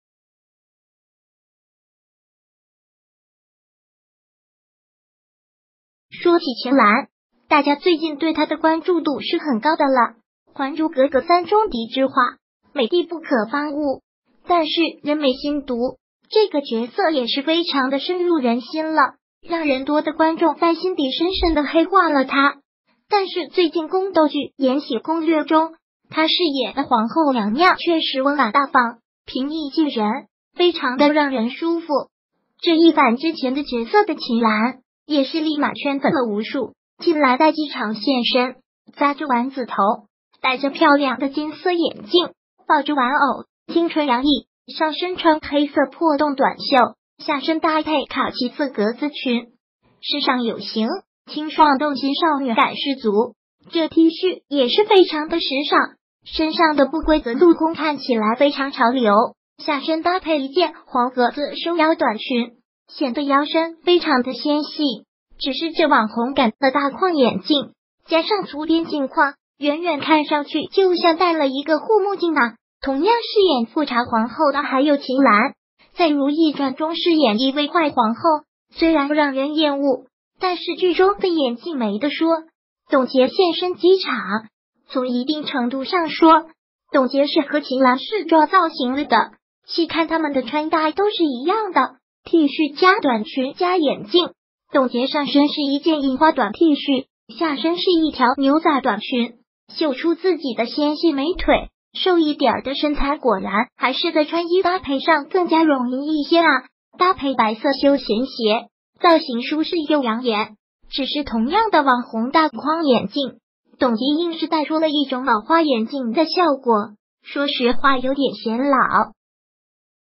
两位“富察皇后”戴网红眼镜，一个戴成护目镜，一个戴成老花眼镜
说起秦岚，大家最近对她的关注度是很高的了。《还珠格格3》中的知画，美的不可方物，但是人美心毒，这个角色也是非常的深入人心了。让人多的观众在心底深深地黑化了她，但是最近宫斗剧《延禧攻略》中，她饰演的皇后娘娘确实温婉大方，平易近人，非常的让人舒服，这一反之前的角色的秦岚，也是立马圈粉了无数。秦岚在机场现身，扎着丸子头，戴着漂亮的金色眼镜，抱着玩偶，青春洋溢，上身穿黑色破洞短袖，下身搭配卡其色格子裙，时尚有型，清爽动心少女感十足。这T恤也是非常的时尚，身上的不规则镂空看起来非常潮流，下身搭配一件黄格子收腰短裙，显得腰身非常的纤细。只是这网红感的大框眼镜，加上粗边镜框，远远看上去就像戴了一个护目镜啊。同样饰演“富察皇后”的还有秦岚，在《如懿传》中饰演一位坏皇后，虽然让人厌恶，但是剧中的演技没得说。董洁现身机场，从一定程度上说，董洁是和秦岚是撞造型了的，细看她们的穿搭都是一样的，T恤+短裙+眼镜。董洁上身是一件印花短T恤，下身是一条牛仔短裙，秀出自己的纤细美腿，瘦一点儿的身材果然还是在穿衣搭配上更加容易一些啊，搭配白色休闲鞋，造型舒适又养眼。只是同样的网红大框眼镜，董洁硬是戴出了一种老花眼镜的效果，说实话有点显老。